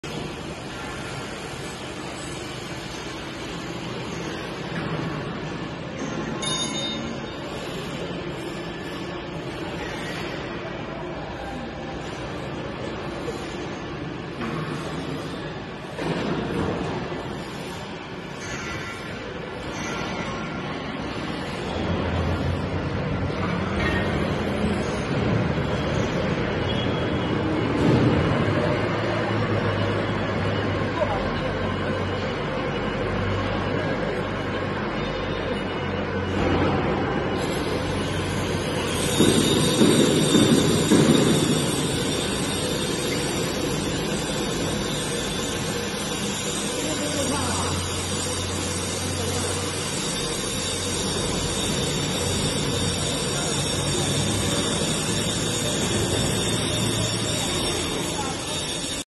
Tobee® 14X12 Pump Impeller G12147 and Volute Liner G12110
G12147A05 Impeller is one of the wear parts for 14/12ST-AH Horizontal Slurry Pumps.

Slurry Pump Impeller is the main component in centrifugal slurry pumps which has vanes to impart and direct the centrifugal force to the slurry.

Slurry pump impeller is also the most diverse component in slurry pumps, by changing impeller, the same slurry pump can cover more different duties.

Slurry pump impellers are normally made in high chromium alloys or natural rubber, special materials are also available.

Tobee® offers three different types of slurry pump impellers: open, closed, and semi-closed. Each has its own strengths and weaknesses, depending on the application. Some are better for solids handling, others are better for high efficiency. To learn more about the specifics of each type of slurry pump impeller.